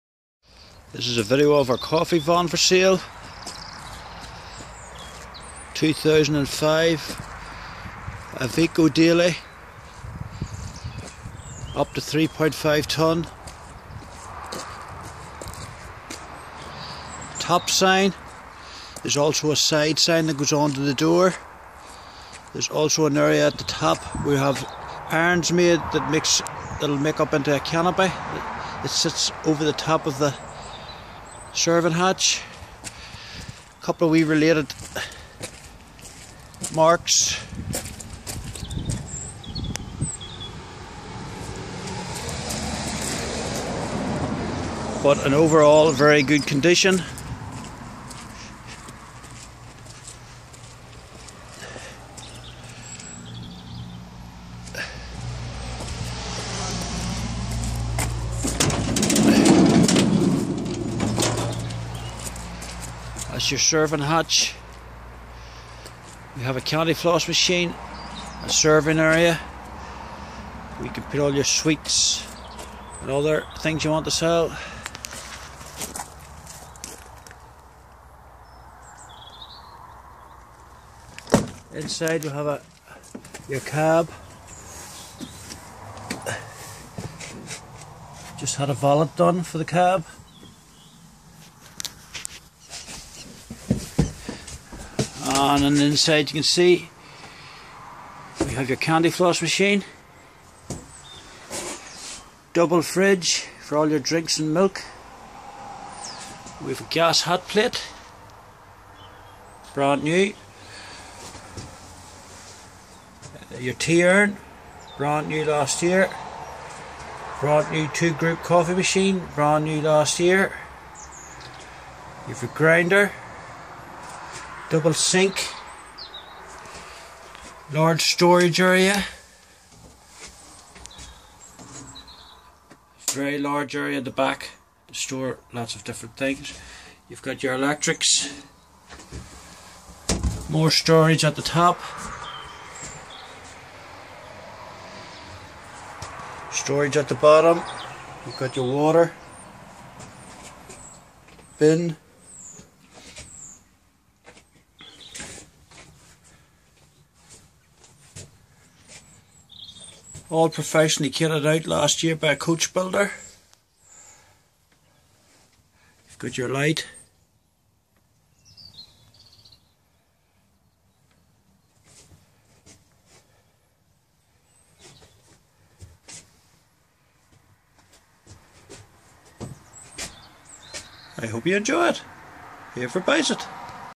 coffee van for sale
2015 coffee van for sale.
Equipped with 2 group coffee machine,fridge,tea urn,sinks,grinder,candyfloss machine.
Runs on gas and two leisure battery's that are charged by solar panel on roof.
Ready to start earning money